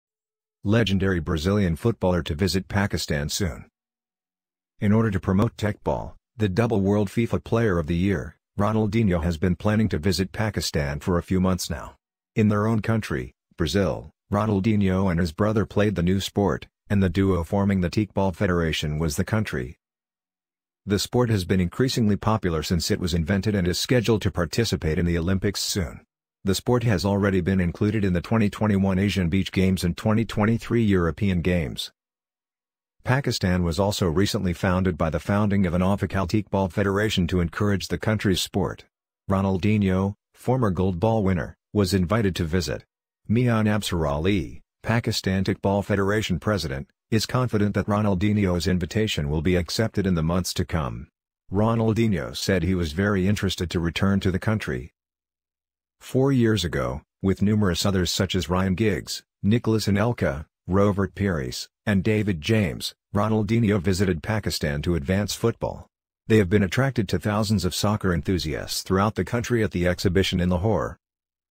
The Public Square| which Legendary Brazilian Footballer to Visit Pakistan Soon???
Two-time FIFA World Player of the Year, Ronaldinho, is set to visit Pakistan in the next few months to promote Teqball. Ronaldinho and his brother have been influential in promoting the new sport in his home country, Brazil, and the duo is responsible for forming the official Teqball federation in the country.